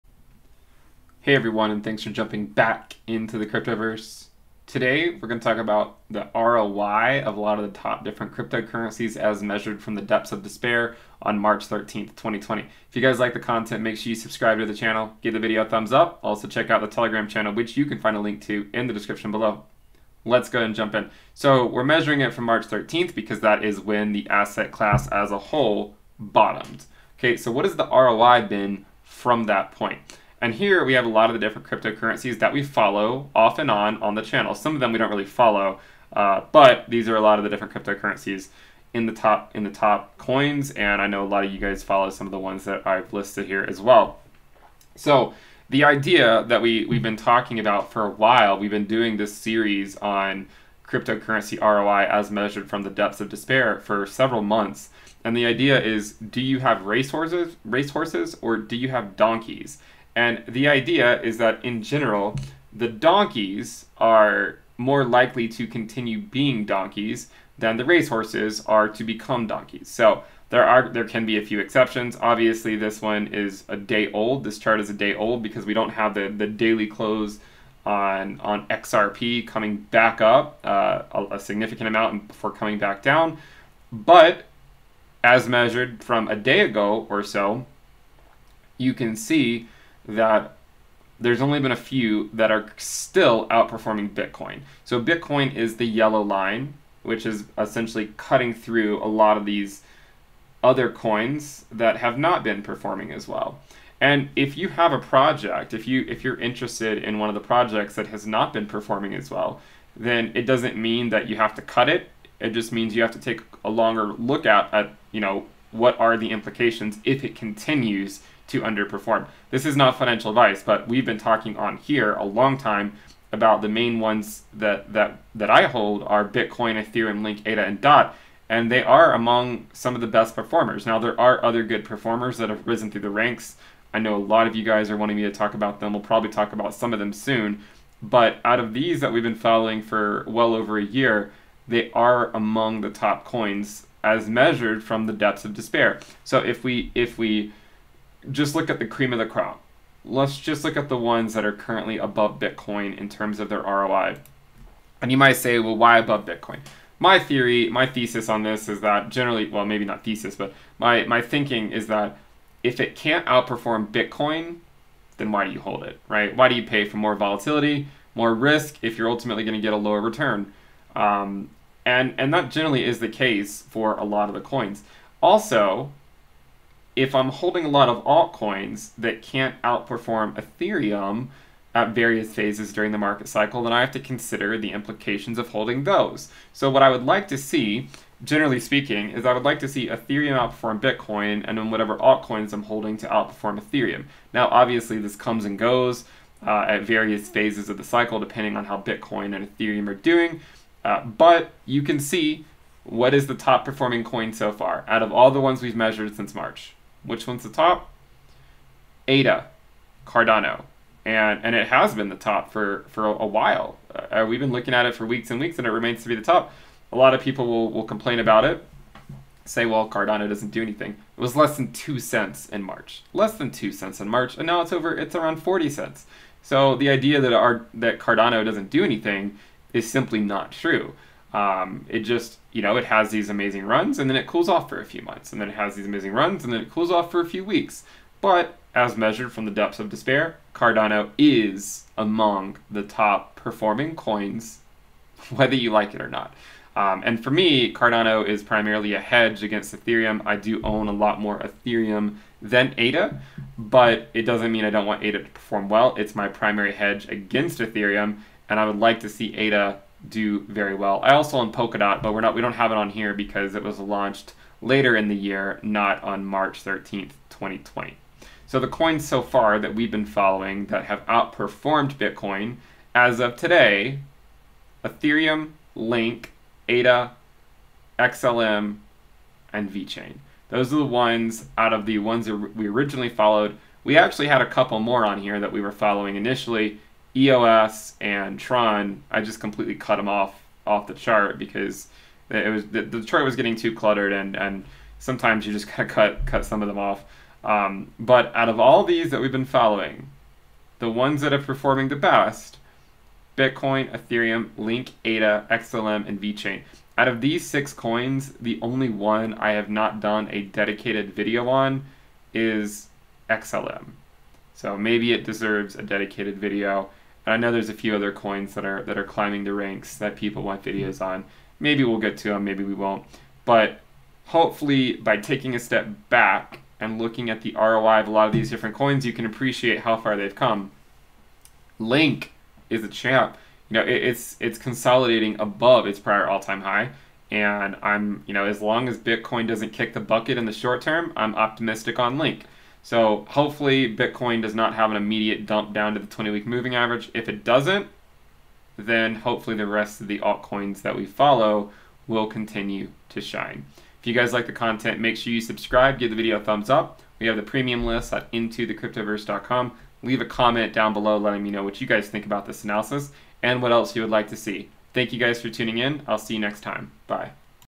Cryptocurrency ROI from the depths of despair
Time for an update on our ROI from the depths of despair (March 13th, 2020). So far, Cardano (ADA) still retains the lead, and is even having a nice move as I made this video. By systematically checking macro-level ROI, we can make sure various coins are still applying themselves in this race, or if they are lagging behind. So far, the main coins we follow are still leading the pack!